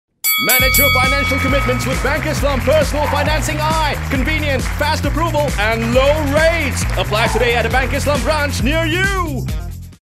Bank Islam Personal Financing-i
Let us help you to simplify all your finances and gain peace of mind and more savings! Free yourself from multiple financial commitments with Bank Islam Personal Financing-i.

Apply today!